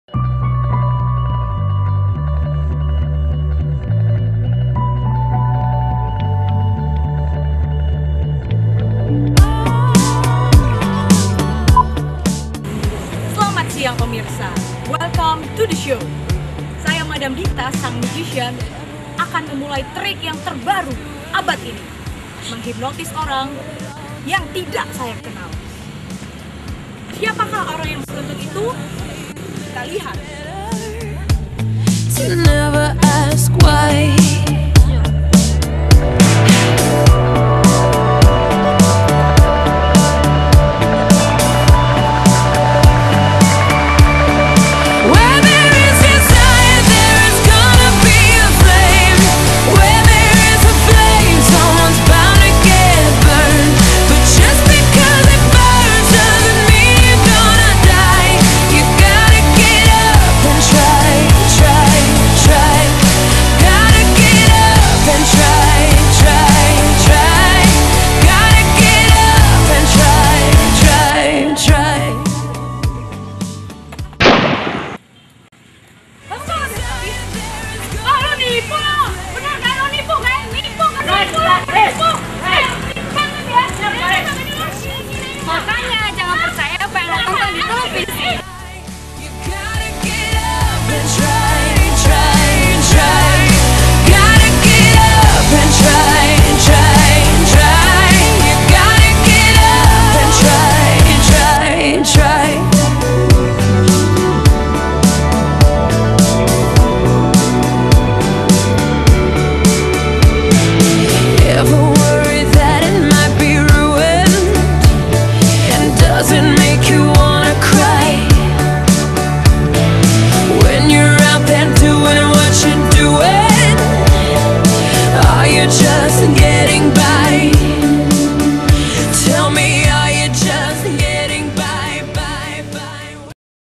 TEORI KULTIVASI (JANGAN PERCAYA APA YANG ANDA TONTON DI TV)
KAMI MENGERJAKAN PROJECT KAMPUS, MEMBUAT FILM / IKLAN / SHORT MOVIE TENTANG SALAH SATU TEORI KOMUNIKASI DAN TERBUATLAH IKLAN INI (TUGAS ADVEL)

NARASI :

SEORANG MAGICIAN TERKENAL "MADAM DITTA" AKAN MULAI MENGHIPNOTIS ORANG SECARA LIVE UNTUK PERTAMA KALINYA. TIBA-TIBA SALAH SATU FANSNYA LEWAT DAN TIDAK SENGAJA MENYENTUH BADAN KORBAN YANG DIHIPNOTIS (DALAM KASUS INI KORBAN ADALAH CREW DAN HANYA BERPURA - PURA TERHIPNOTIS) KARNA AKHIRNYA TAU PESULAP FAVORITNYA MENIPU, DI FANS TADI LANGSUNG EMOSI DAN MAU MEMUKUL MADAM DITTA.

INTINYA JANGAN PERCAYA APAPUN YANG ANDA LIHAT DI TV KARNA SEMUA BISA SAJA SKENARIO